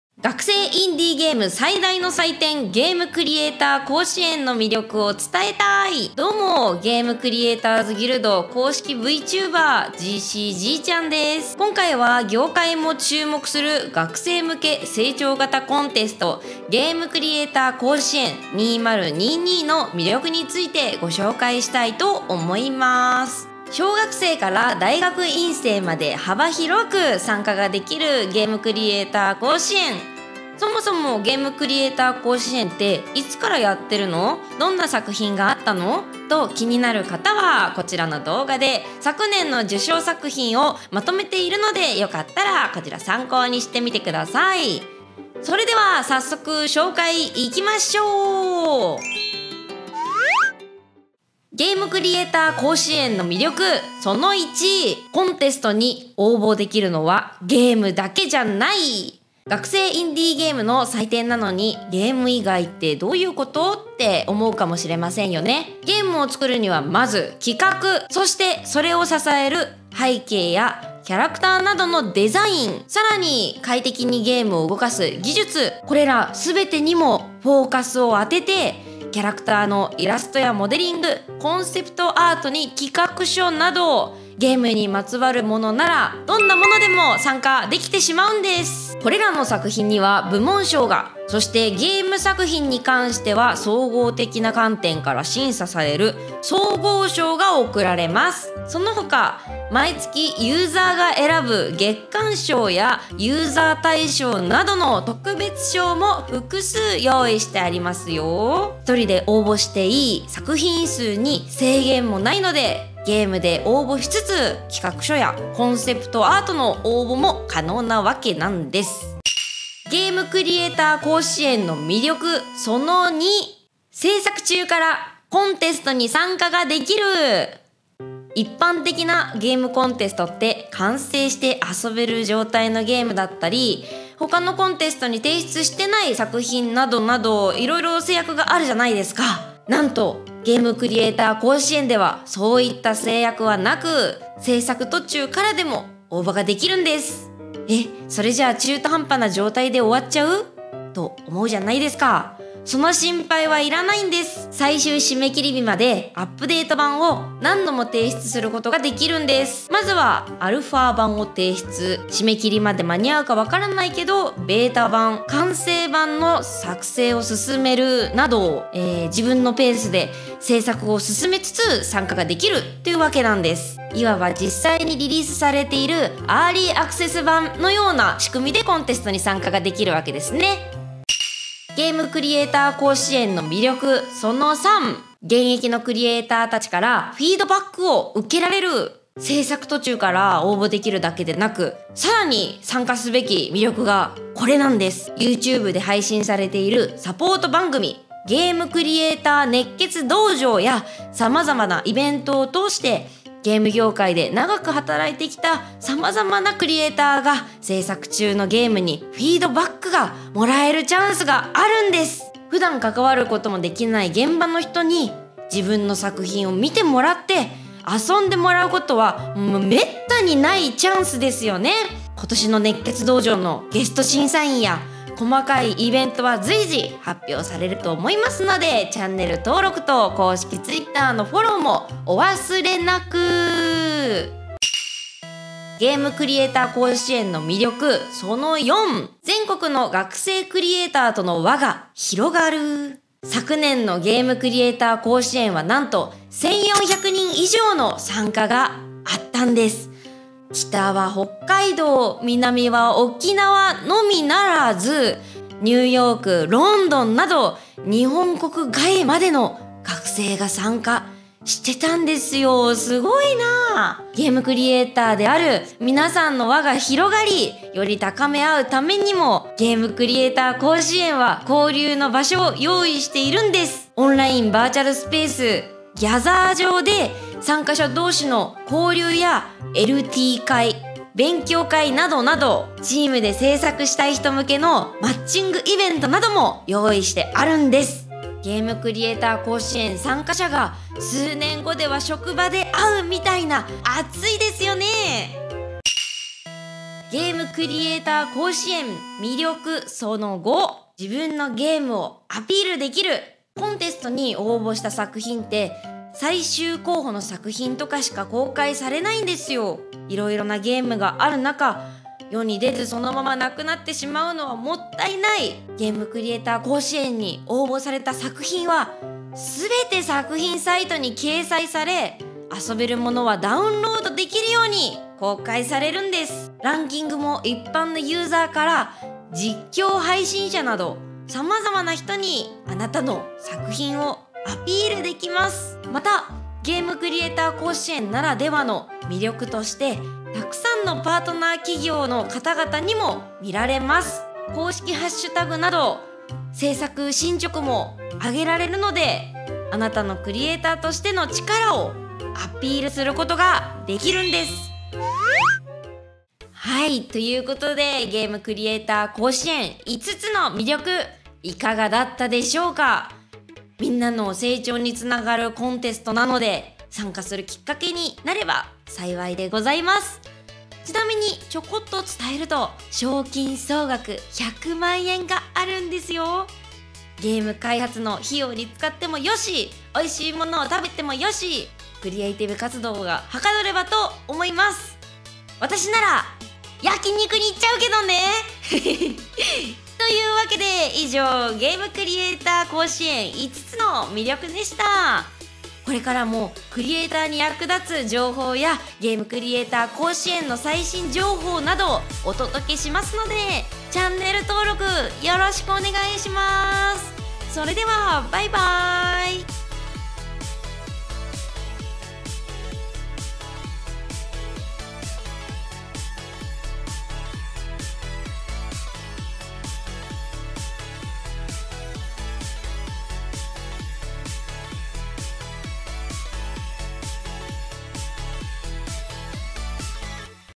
ゲームクリエイター甲子園に参加するだけで受けれるメリットを紹介 【ゲーム開発】
今年も学生作品が集まる！

🎮やってほしいゲームとか質問とか募集中！